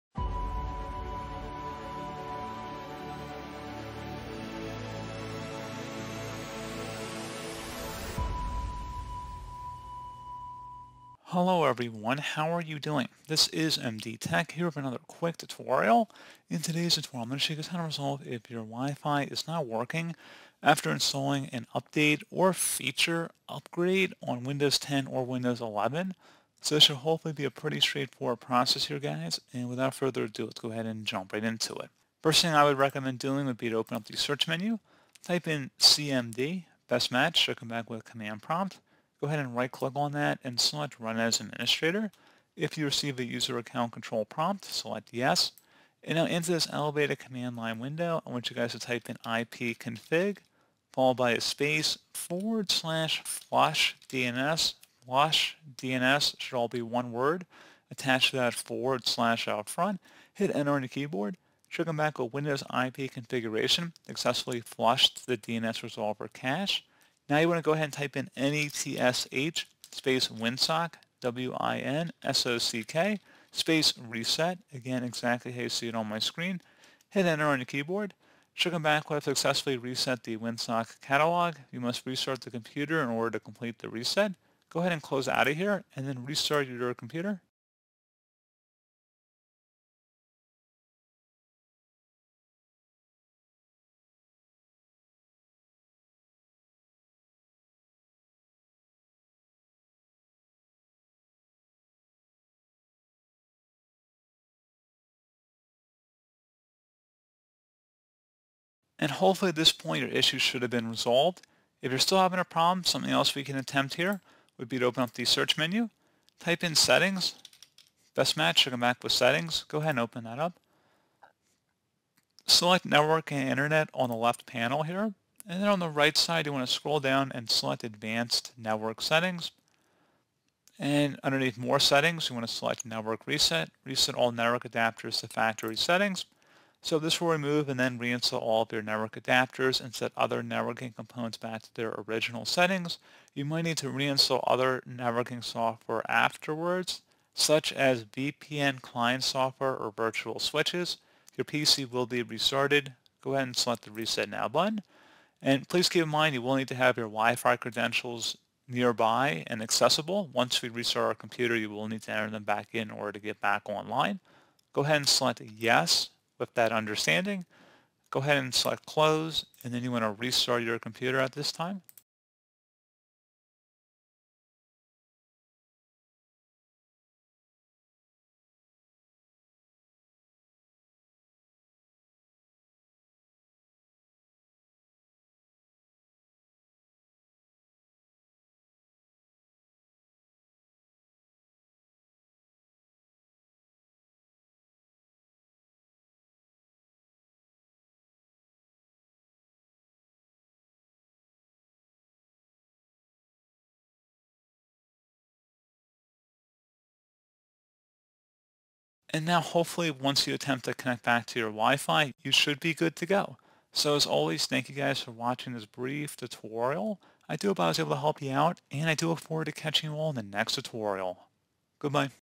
How to Fix No WiFi After Windows Update in Windows 11 - [Solution]
How to Fix No WiFi After Windows Update in Windows 11

On Windows 11, Wi-Fi connectivity is one of the most common problems. You always come across user reports complaining about slow Wi-Fi speeds, and error messages like “Limited access,” “Unidentified Network,” or no internet access.

Your Windows 11 computer may not show a Wi-Fi connectivity option due to various reasons. Corrupt Wi-Fi drivers, hardware issues, and even problems with the Windows OS can be the potential causes for this problem.

In addition, you may also notice frequent Wi-Fi connection drops. If so, here are a few troubleshooting steps to help you fix the Wi-Fi not working problem in Windows 11.

If your device has networking issues, Windows 11 has a feature to reset Wi-Fi and Ethernet network adapters to fix the most common connectivity problems, even slow Wi-Fi speeds on Surface and other computers.

On Windows 11, Microsoft proactively releases updates to patch bugs, improve security and performance, and occasionally add new features. However, sometimes, system updates can also cause unwanted problems, and many times are connectivity issues.

For example, a lot of times, updates can make speeds and connections unreliable, especially with wireless adapters. Also, they can break connectivity entirely to the network and internet, cause network driver problems, and more.